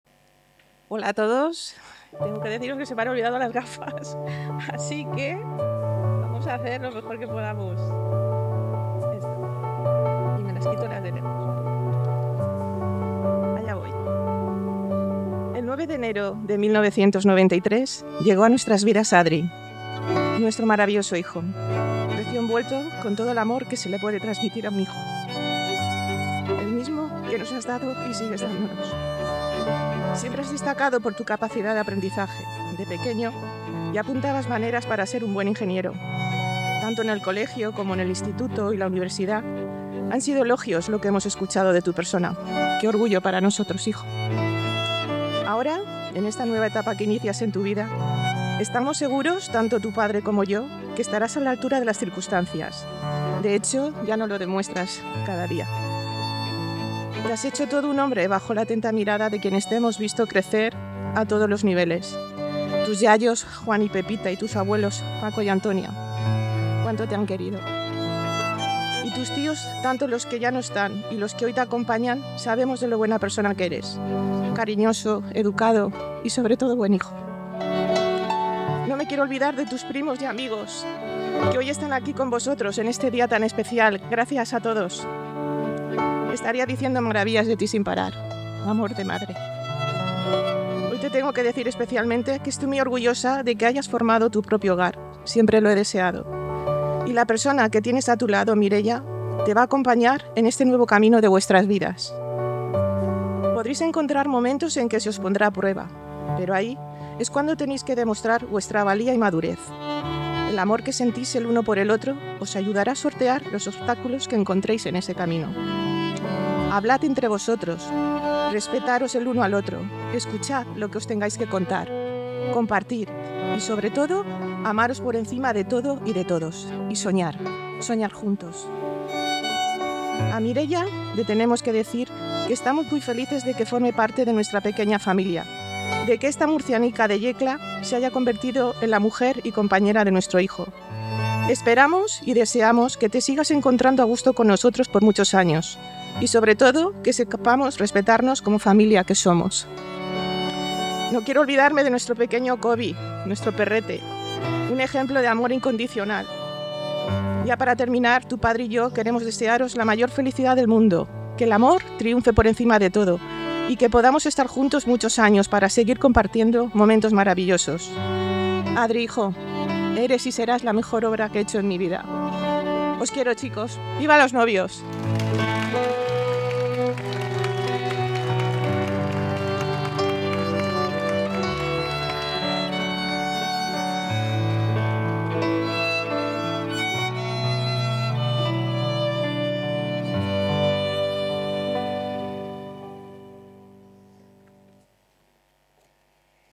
Emotivo Discurso de Boda de la Madre de Adrián | Valores y Buenos Momentos en Familia
Este discurso de boda de la madre de Adrián es un verdadero homenaje a los valores y al amor incondicional. En sus palabras, ensalza no solo los logros académicos de su hijo, sino también sus cualidades afectivas, destacando lo orgullosa que se siente de la persona en la que se ha convertido. Este emotivo mensaje refuerza la importancia de pasar más tiempo juntos y aprovechar cada instante en familia.

La boda de Mireya & Adrián en la Finca El Teular, en Sueca , fue un escenario perfecto para recordar el valor de los buenos momentos, de esos instantes que refuerzan los lazos familiares. Este discurso es un hermoso recordatorio de la conexión entre madre e hijo y del poder de la familia en los momentos más importantes de la vida. Si buscas inspiración o quieres recordar la magia de los vínculos familiares, ¡dale al play y acompáñanos en este homenaje!